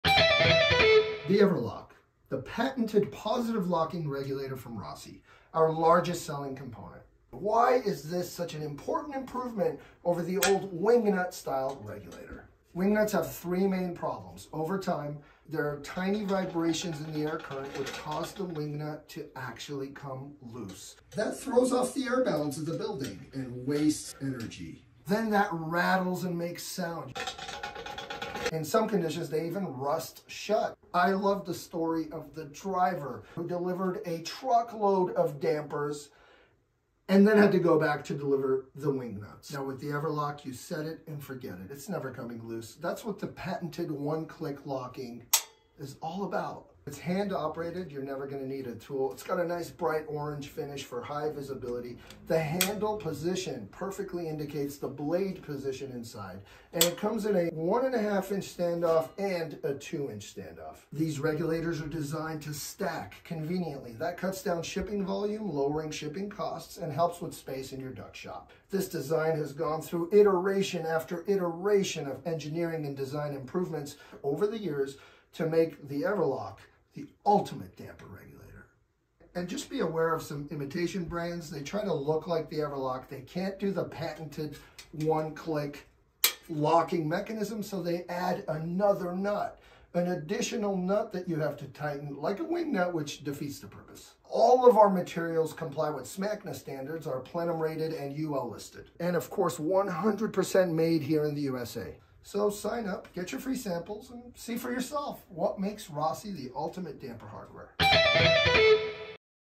Everlock - patented self-locking regulator
This video showcases the specific features of the patented self-locking regulator from Rossi, the Everlock, which is taking over the industry and replacing wingnuts.